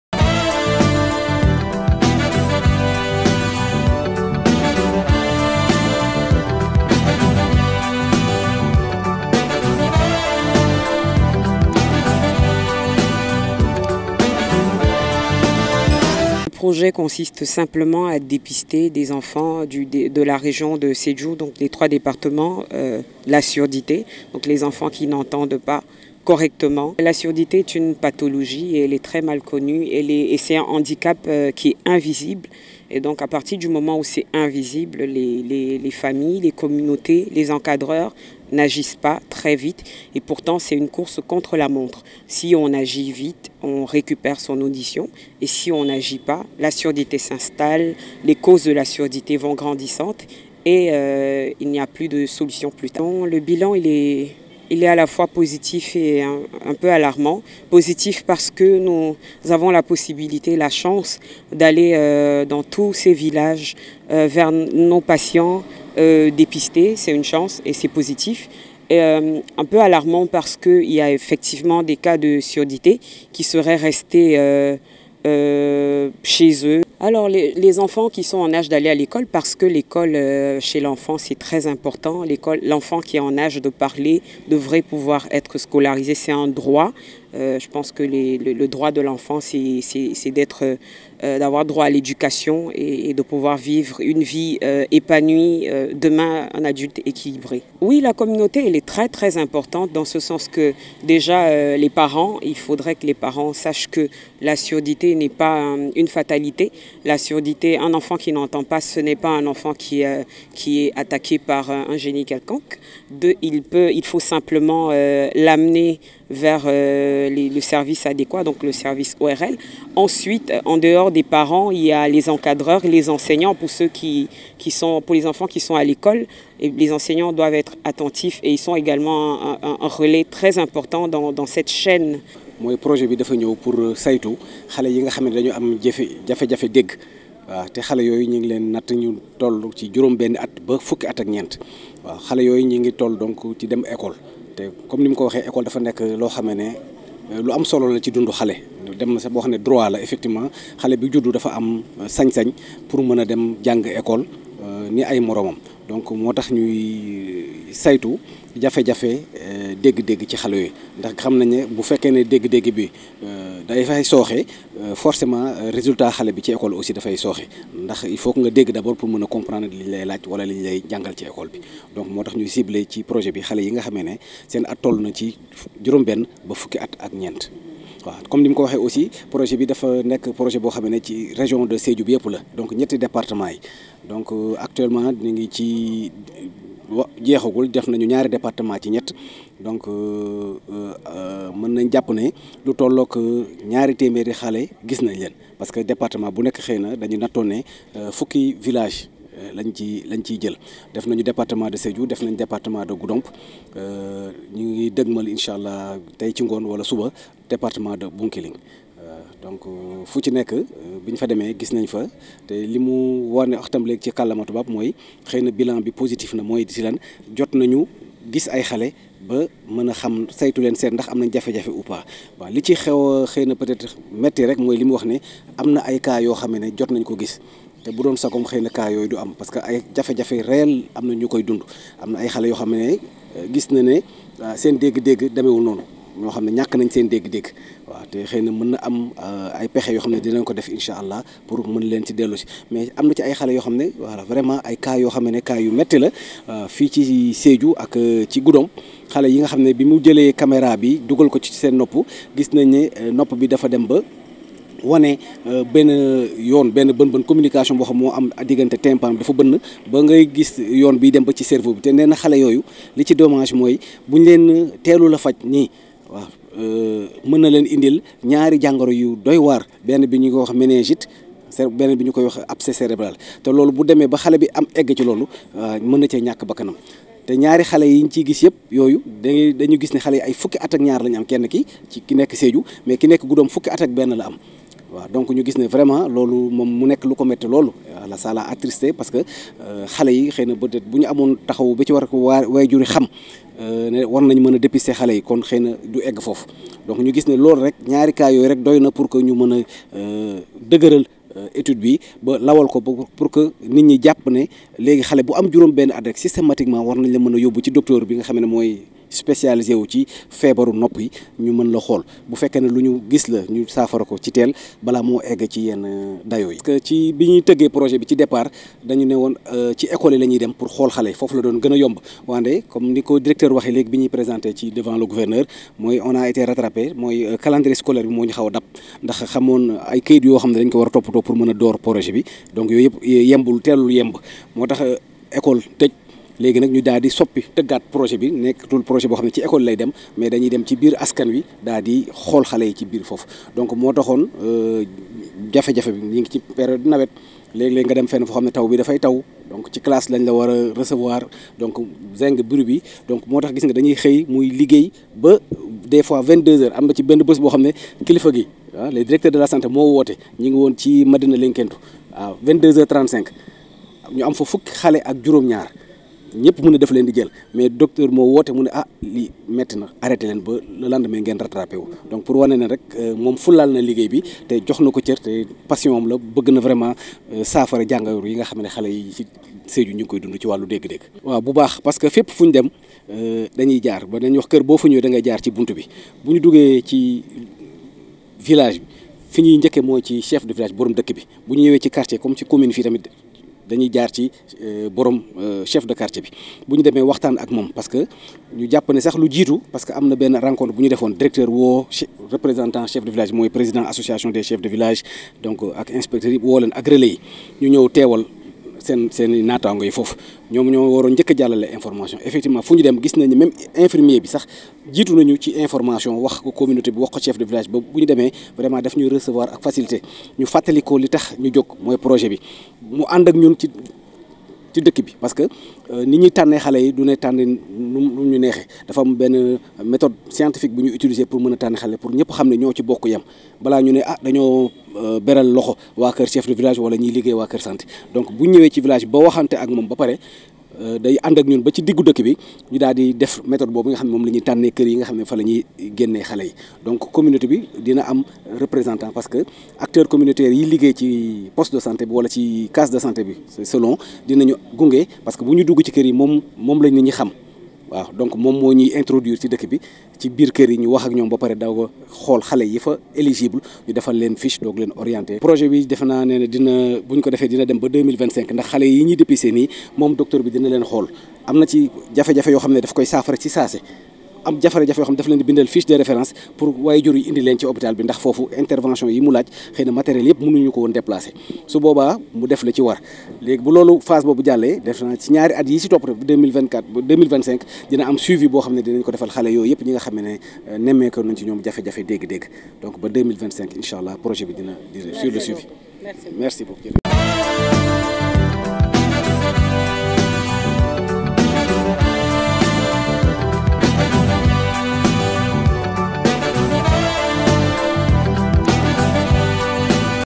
Dépistage des enfants sur la surdité: "une course contre la montre à Sédhiou", selon Dr Ndaly
Bienvenue sur la page officielle DEM TV. Retrouvez sur cette chaîne You Tube nos meilleurs Interviews, reportages, publireportages, directs et émissions.